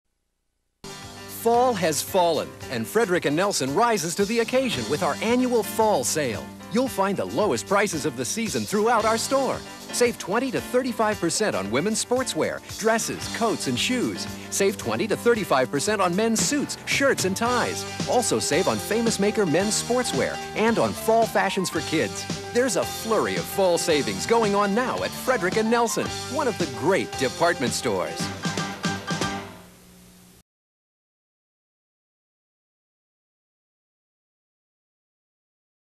Frederick and Nelson Store Commercial 1987
Frederick & Nelson was a big thing in Seattle for almost a century.